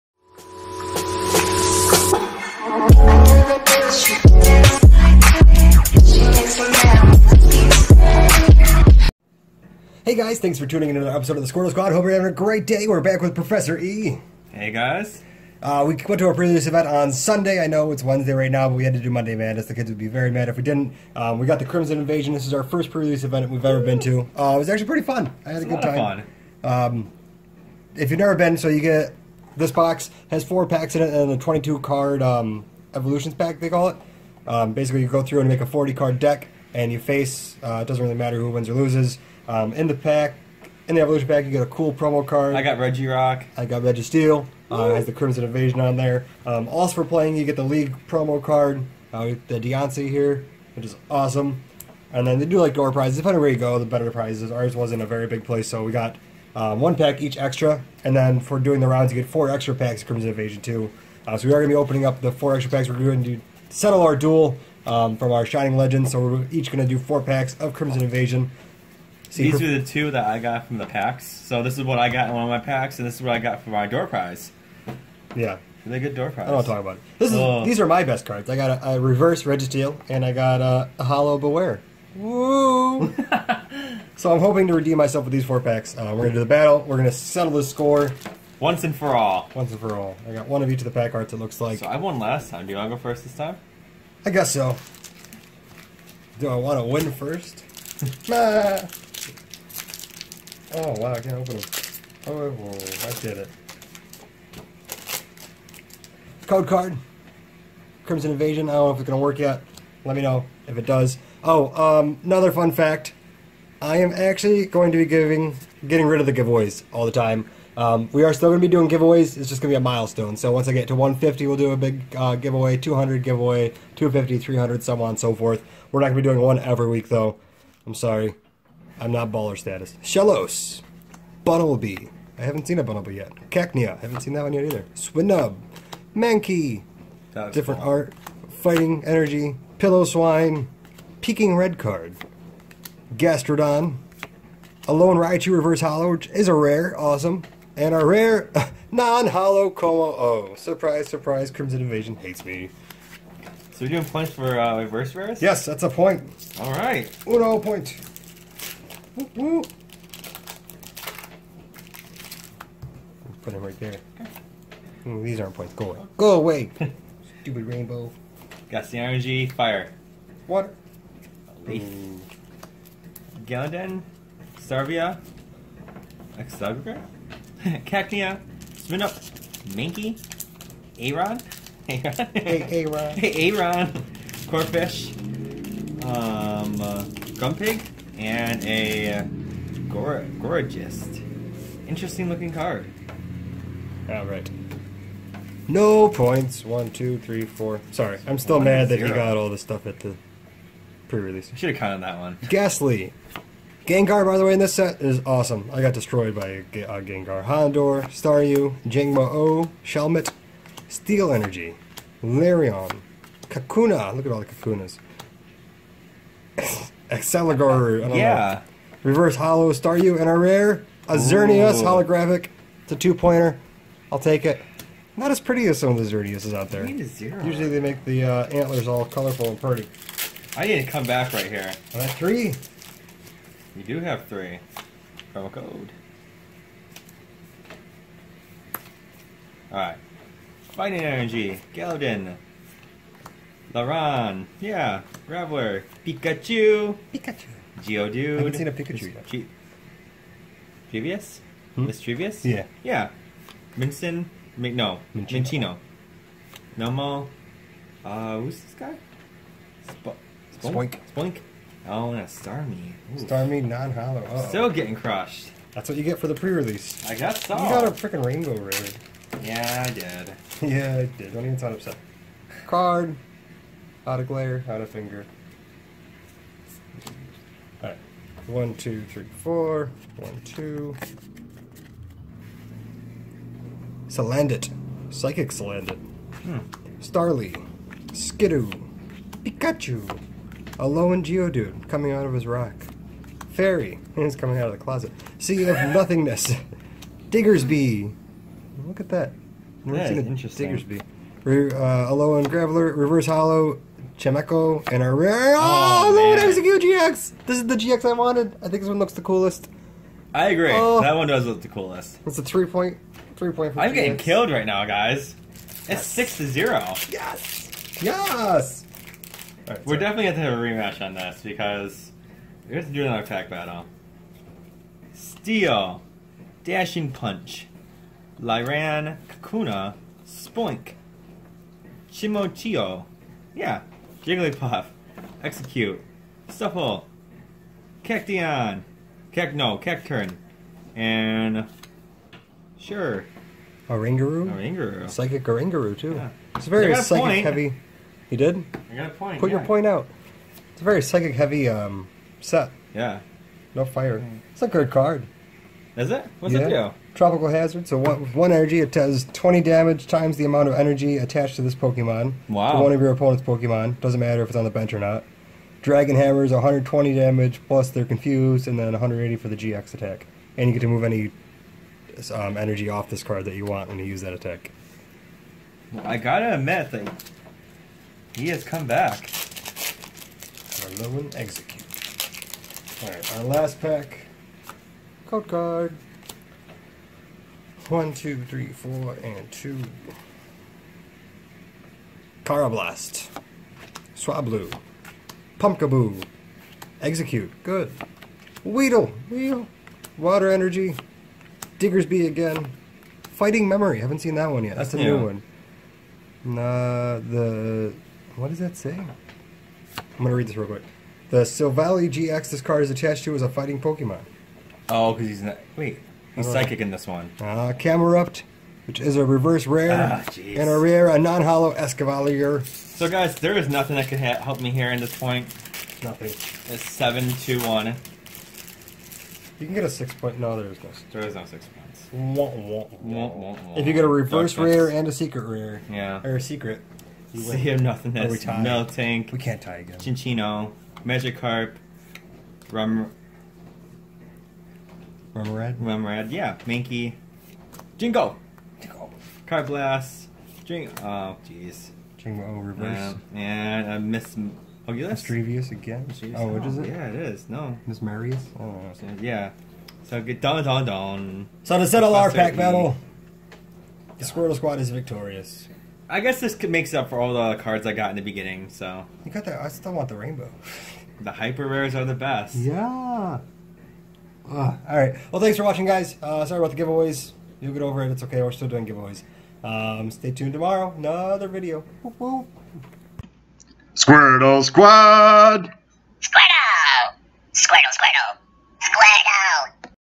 Crimson Invasion Final Battle Against Professor E To See Who The Real Winner Is? PRE RELEASE!!
Professor E comes back to see who the winner of our tie breaker is from our shining legends pack battles! We will be using four packs each of Crimson Invasion that we got from a pre release event on Sun.
As stated in the video we will not be doing give aways every Saturday anymore. WE WILL STILL DO THEM ON MILESTONES 150 SUBS, 200 SUBS, ETC.

Message or comment if you want to do a pack battle, would love to collaborate with someone!